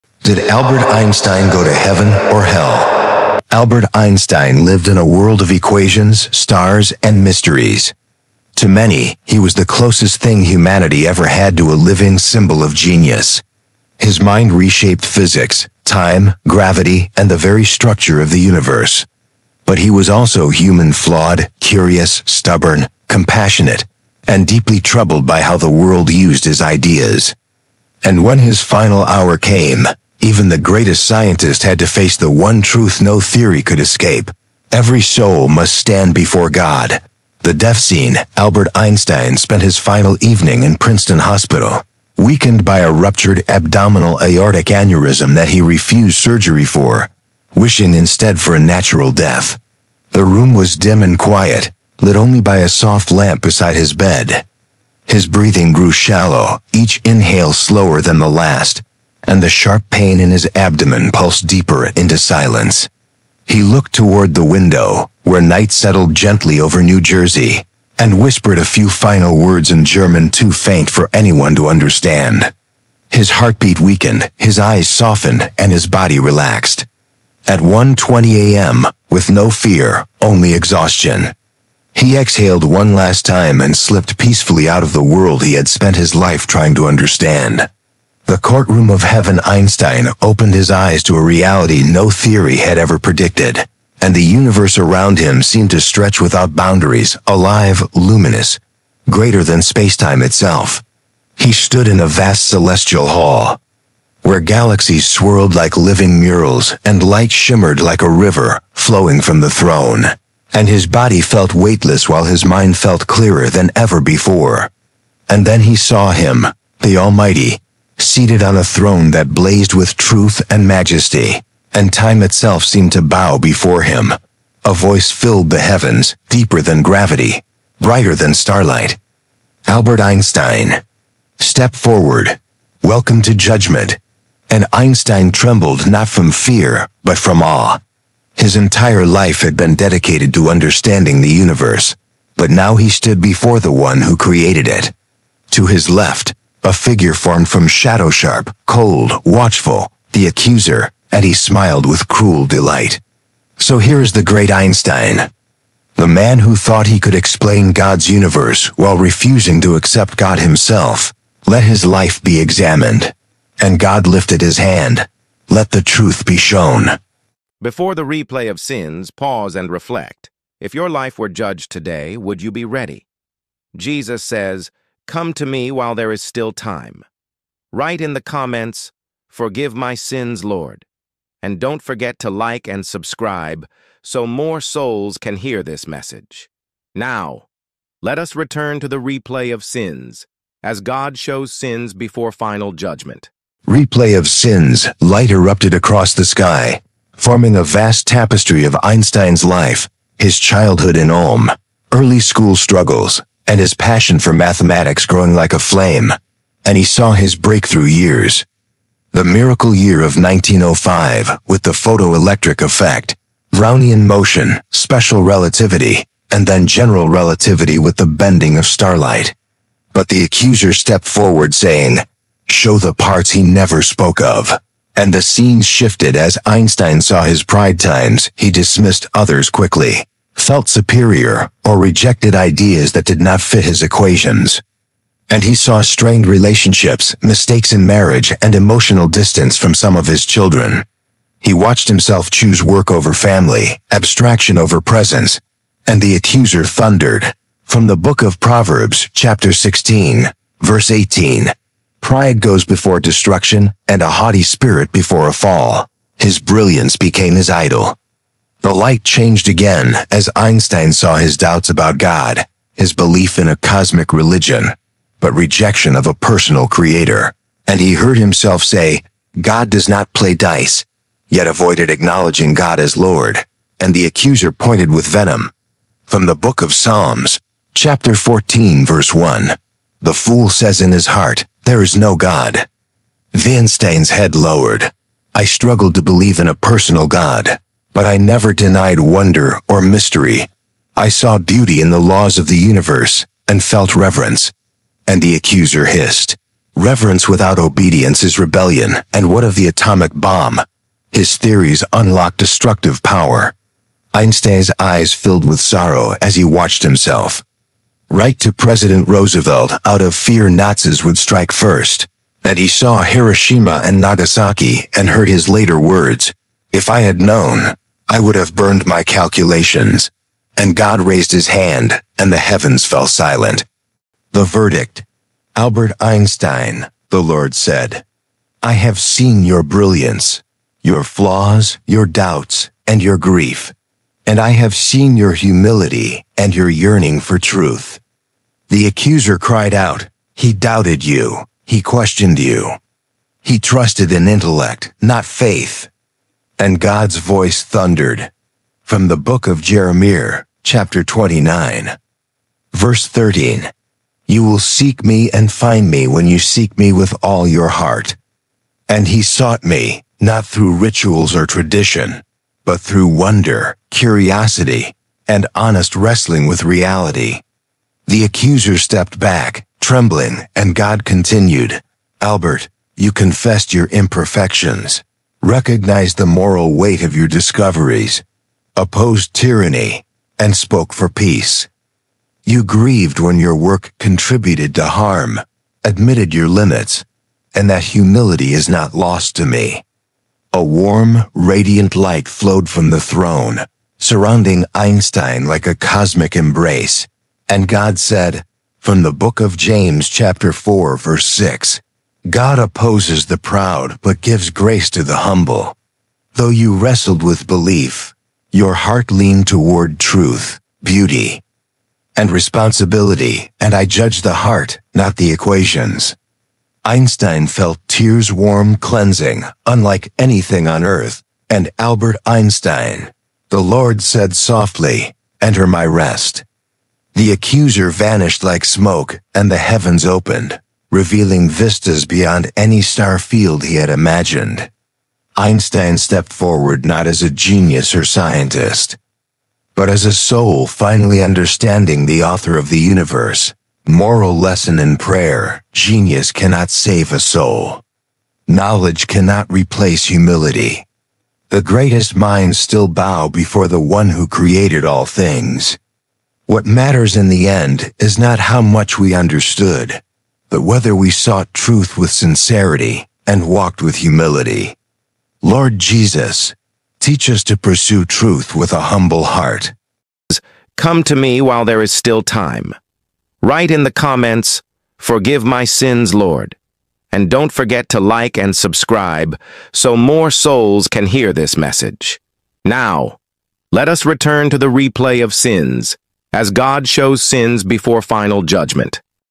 Did Albert Einstein Go to Heaven or Hell?
Albert Einstein is one of the most influential minds in human history— a physicist whose theories changed the world forever. From the miracle year that transformed science to the personal struggles and quiet moments behind his genius, this video explores the real man behind the legend. Discover the brilliance, the humanity, and the lasting impact of Albert Einstein’s life and work.